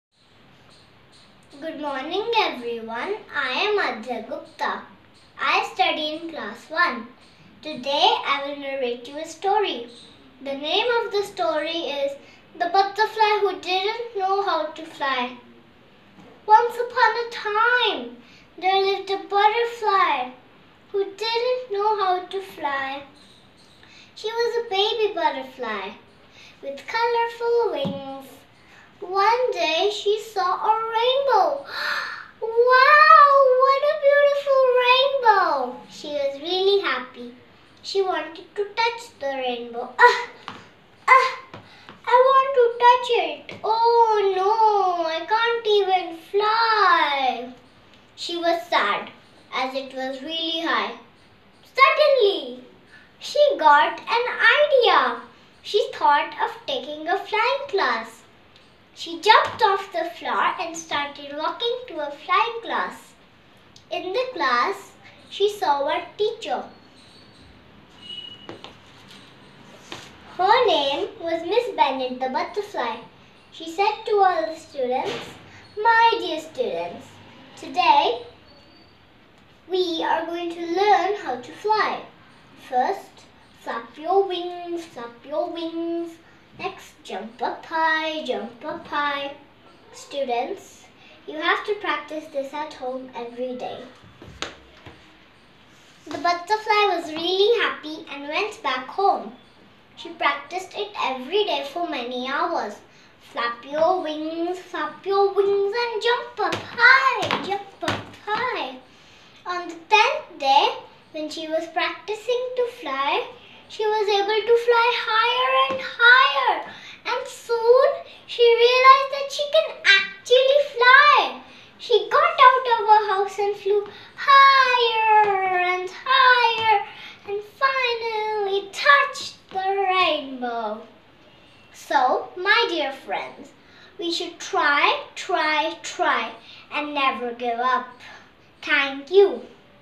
Velocity Unlimited 2020
Child Name - Aadhya Gupta
Class 1
Event - My Story ... My Ways - Once Upon A Time - Story Narration
School Name - Mother's Global School, Preet Vihar, Delhi 92